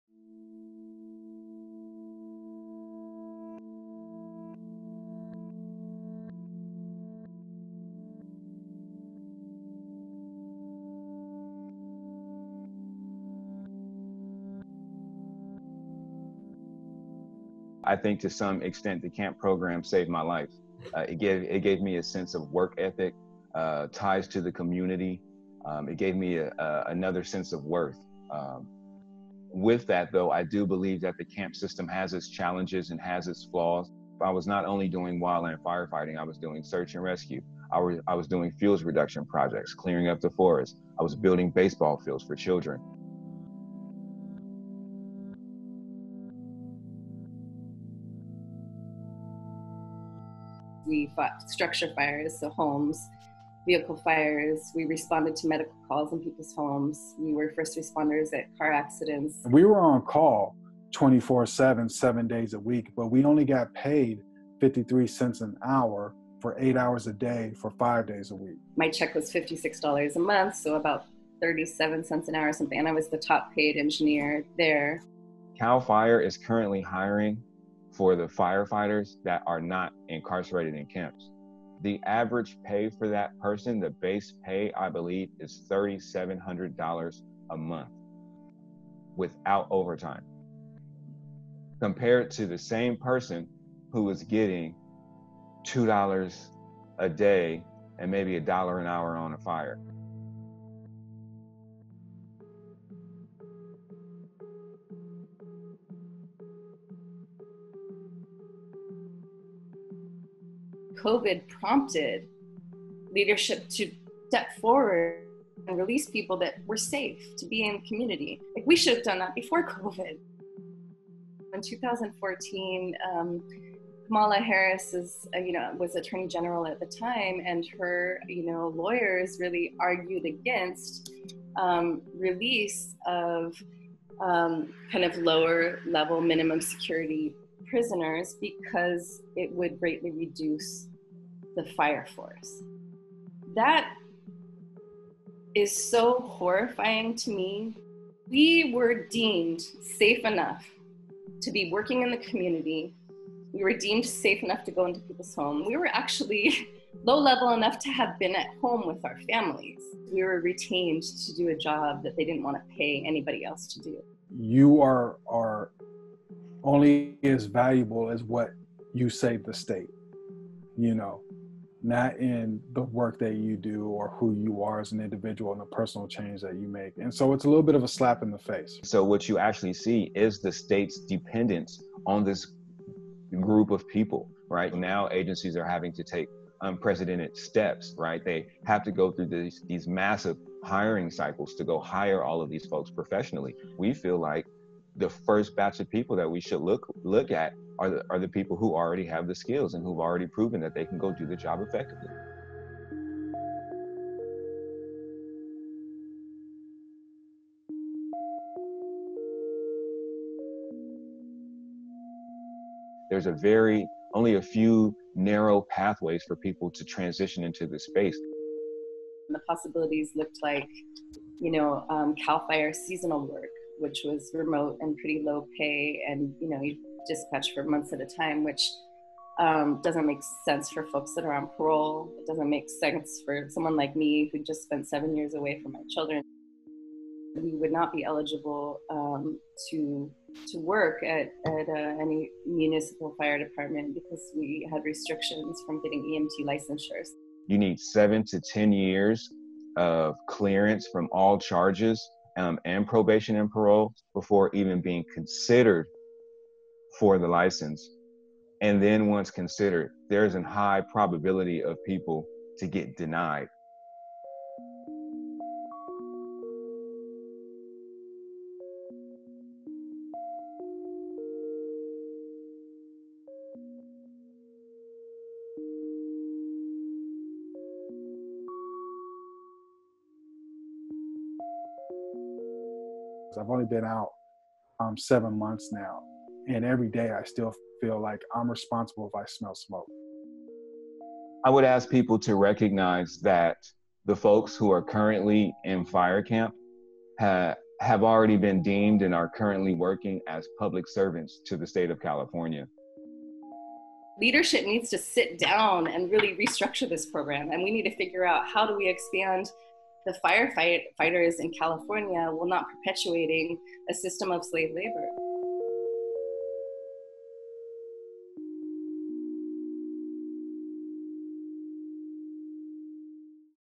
Wildfires: Formerly Incarcerated Firefighters In California On What Needs To Change | TIME
As California faces a historically destructive wildfire season, a firefighter shortage has brought renewed attention to the state’s dependence upon prison labor in battling the blazes. Now, some formerly incarcerated firefighters are calling for a review into how California values its prison firefighters.

California Wildfires : Formerly Incarcerated Firefighters On What Needs To Change